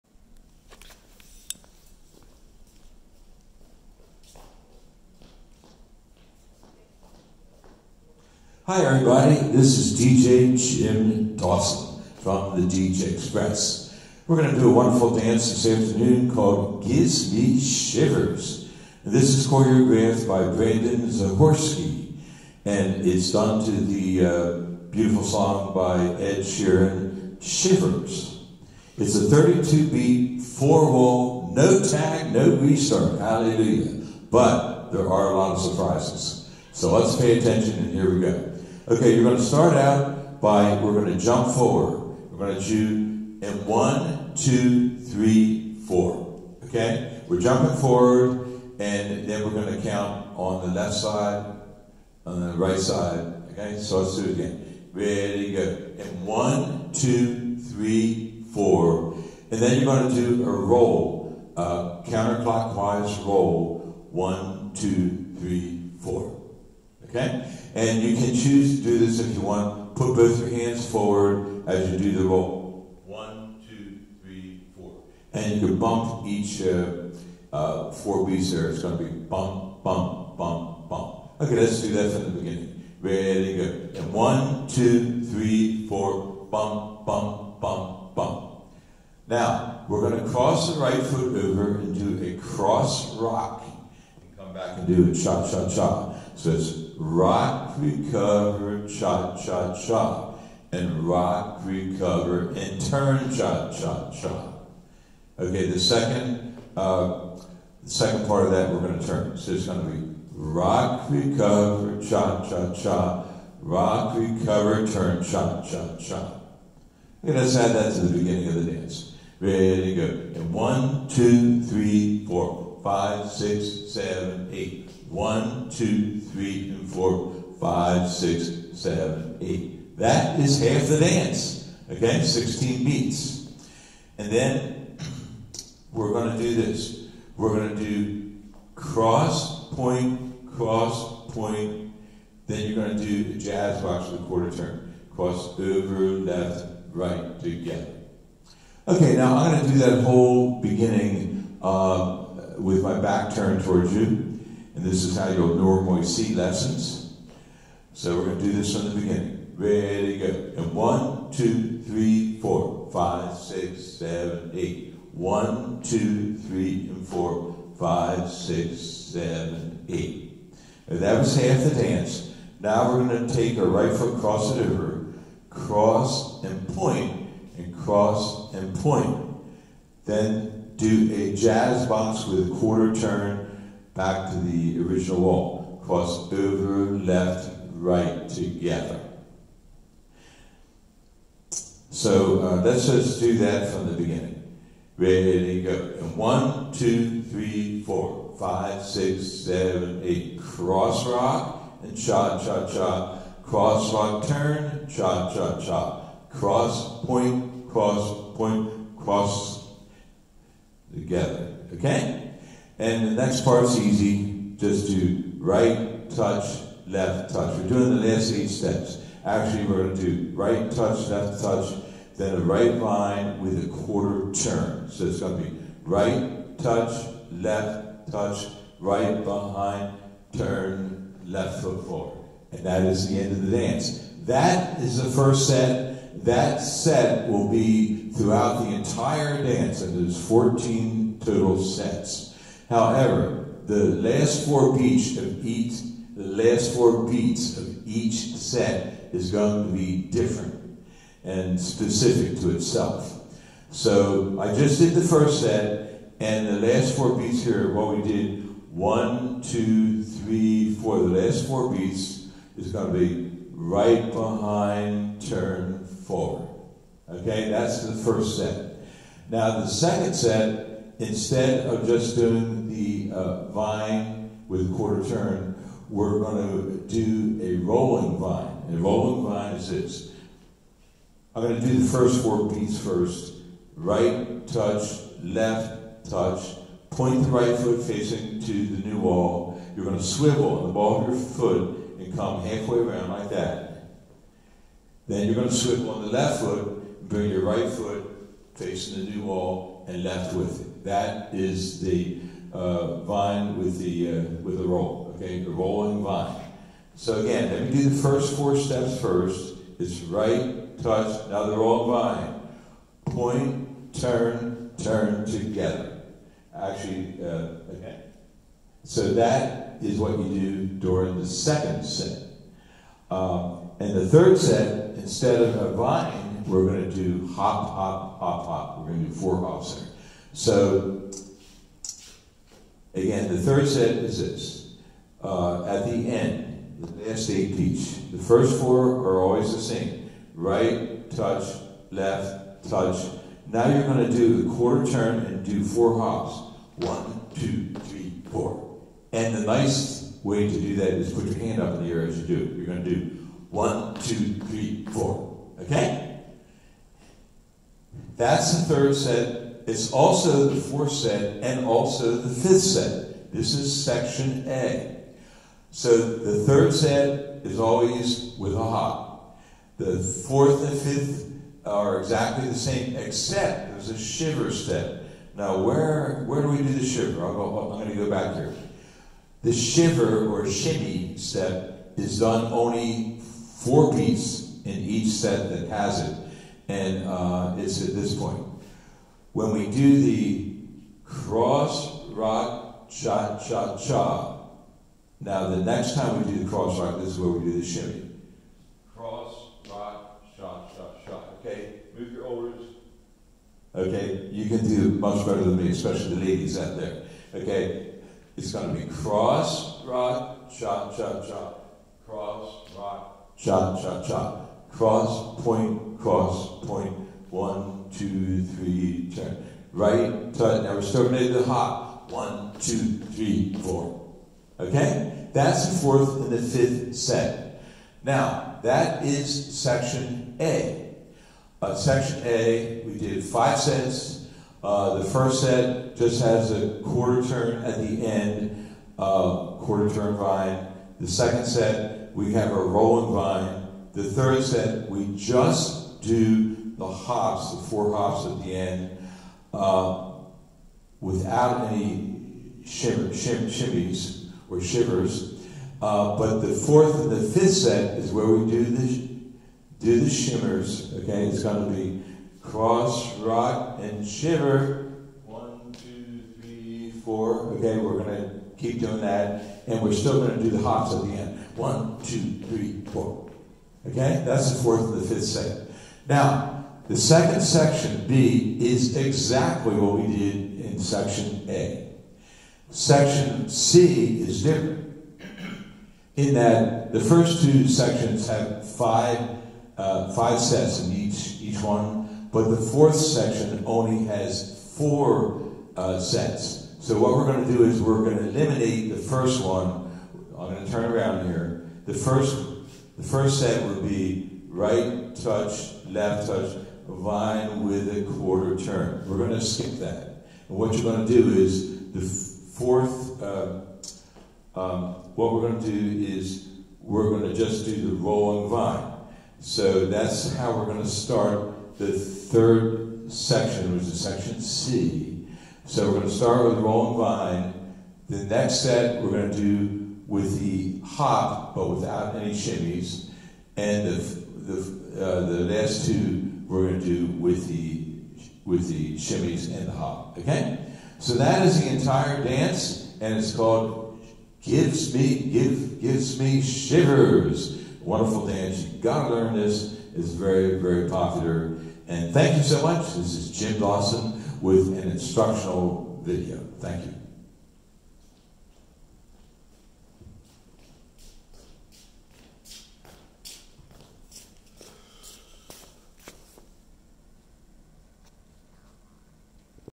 Gives Me Shivers-Instructional Video by Jim Dawson
Name of Dance: Gives Me Shivers

Instructional Video by Jim Dawson

Choreographer: Brandon Zahorsky

No Tags, No Restarts, but with "Surprises"